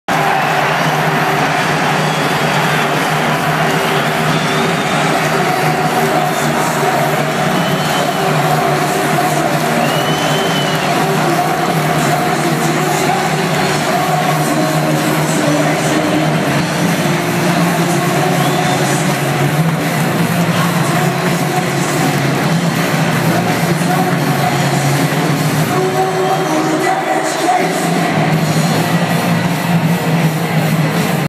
Carling Cup final 2012
Cardiff City and Liverpool take to the pitch at start of epic Carling Cup final 2012